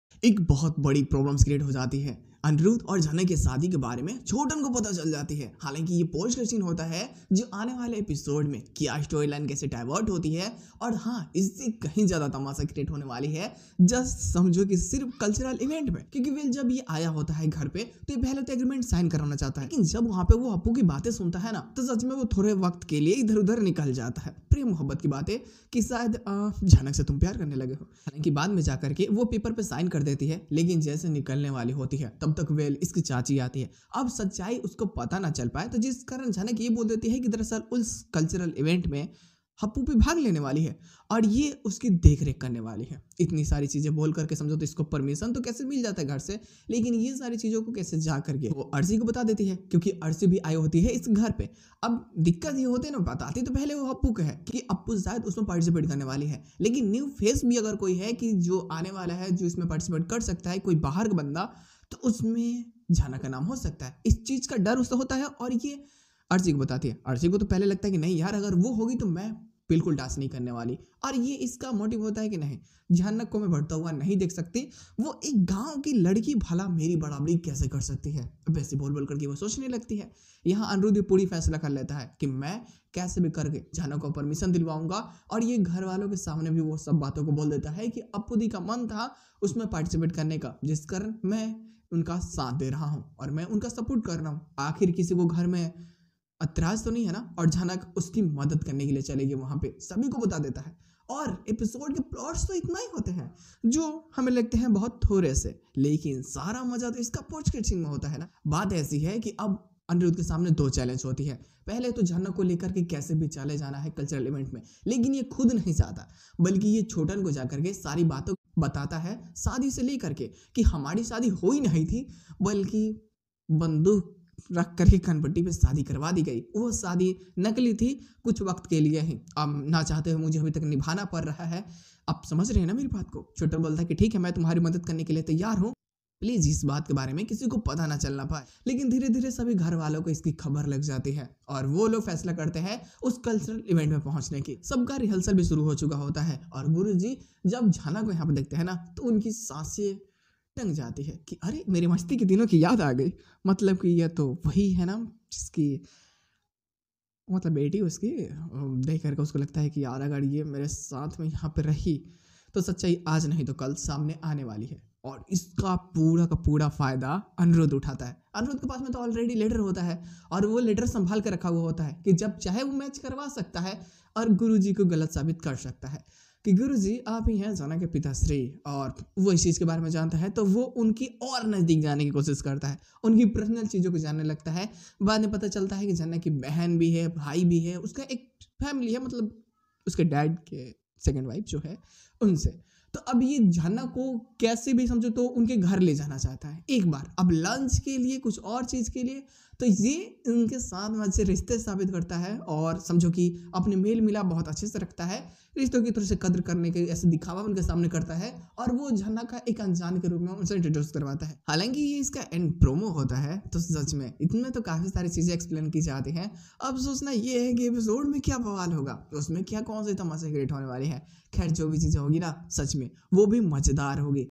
Fight For Rights| Review Video
Anirudh tells Chotan the truth about his marriage and Chotan thinks of helping him but now Anirudh puts full pressure on Guru ji to reveal the truth and he wants Guru ji to tell everyone the whole truth.

About Me
Girie reaction Is a "reaction channel" here you get reaction and funny videos

Key Moments
0:02 Chhotan Knows Anirudh's Marriage Truth
0:08 Video Topics
0:16 Jhanak's Love
0:30 Hapu Participate In Cultural Events
0:50 Arshi Is Angry
1:31 Anirudh Takes Permission for Event
1:42 End Promo Twist
1:56 Chhotan Helps Anirudh
2:26 Guruji Is Shoked
2:46 Anirudh's New Plan